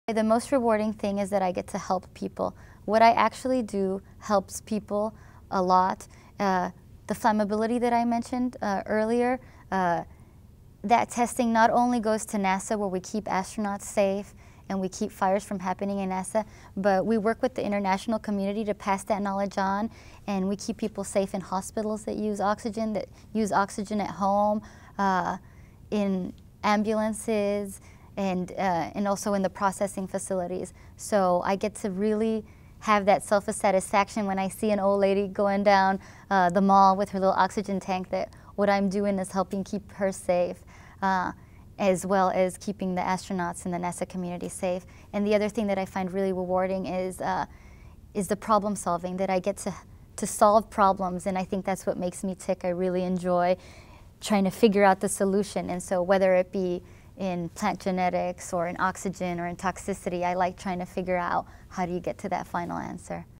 Chemical Engineer: Great Part - Susana Tapia Harper Career Girls Role Model
Chemical Engineer Susana Tapia Harper at NASA shares valuable chemical engineering career guidance and life advice with girls.